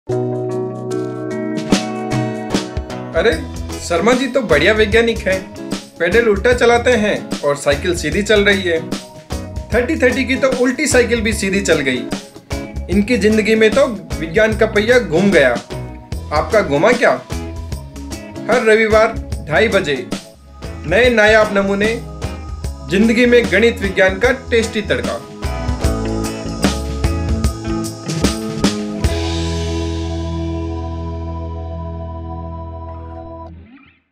CCL IIT Gandhinagar के Sharma जी की Front Back Cycle
Join us as we explore unbelievable cycles, and mysteries of wheel, Sunday at 2:30 PM only

This Sunday we will explore cycles. Even today, scientists are not sure about the physics behind the stability of a bicycle. So join us in exploring Bicycle, History, Math Science and MURDER MYSTERY.

About Us

Say hello on Facebook: goo.gl/f3a1Yo